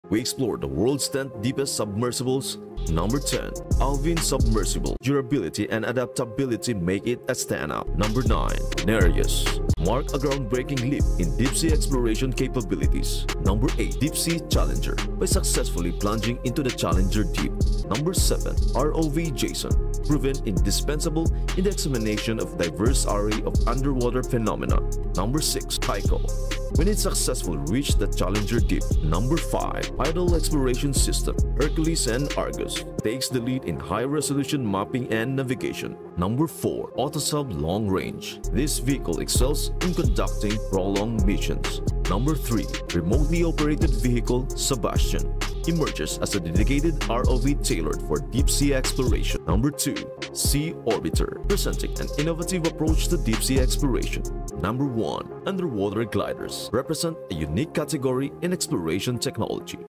The Secret World of Top 10 Underwater Vehicles
Dive into the secret world of the top 10 underwater vehicles! Explore the most advanced submarines and underwater drones that are pushing the boundaries of marine exploration. From deep-sea research vessels to cutting-edge submersibles, uncover the fascinating technology and capabilities behind these underwater marvels. Whether you're curious about oceanic exploration or fascinated by underwater technology, this video reveals the incredible machines shaping the future of underwater discovery!

Timestamps:
0:00 - Intro
0:31 - Alvin Submersible
1:25 - Nereus
2:35 -  Deepsea Challenger
3:27 - ROV Jason
4:27 - Kaiko
5:27 - Hadal Exploration System (Hercules and Argus)
6:29 -  Autosub Long Range
7:28 - Remotely Operated Vehicle (ROV) SuBastian
8:31 - SeaOrbiter
9:28 - Underwater Gliders
10:26 - Outro